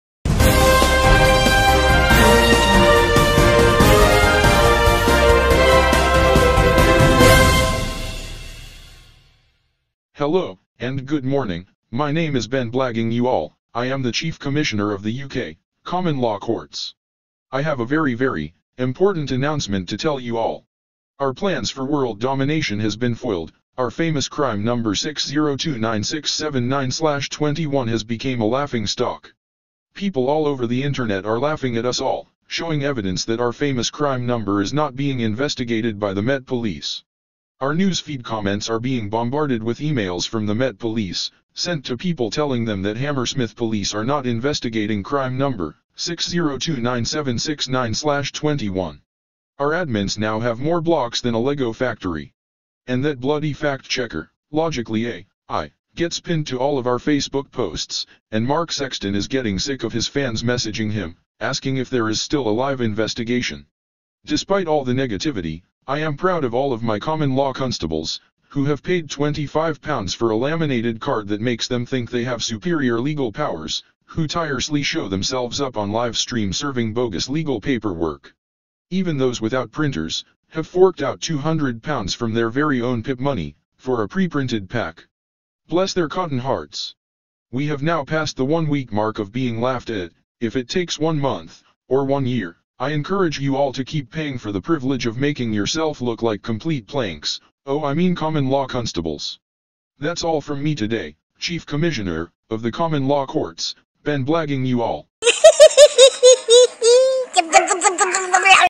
Common Law Courts Chief Commissioner BEN BLAGGINUALL Crime Ref 6029679/21
Parody of the common law courts Chief commissioner BEN BLAGGINUALL who is filling his pockets through the the stupidity of people who sign up to a website to become a CommonLawConstable & pay £25 for a laminated card that they think gives them some sort of superior legal power, IT DON'T!
These Common Law Constable have been serving fictional bogus legal paperwork to police stations all over the UK, Despite it being well known since 12/01/2022 that Hammersmith Police are not investigating crime number 6029679/21